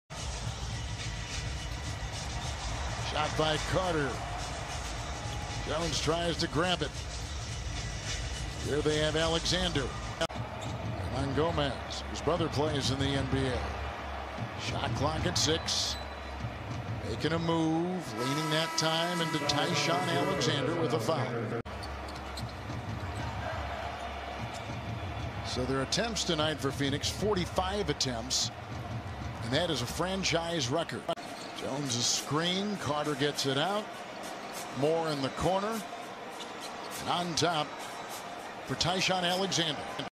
Ty-Shon Alexander : All Possessions (2020-12-30)
Like and sub for more highlight videos. Comment if you want a specific player.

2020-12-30 Phoenix Suns vs New Orleans Pelicans: Player Highlights Ty-Shon Alexander
    0 PTS, 1 REB, 1 AST, 0 STL.
        0 of 1 from 3.
        0 of 2 FGs in 3:54 minutes.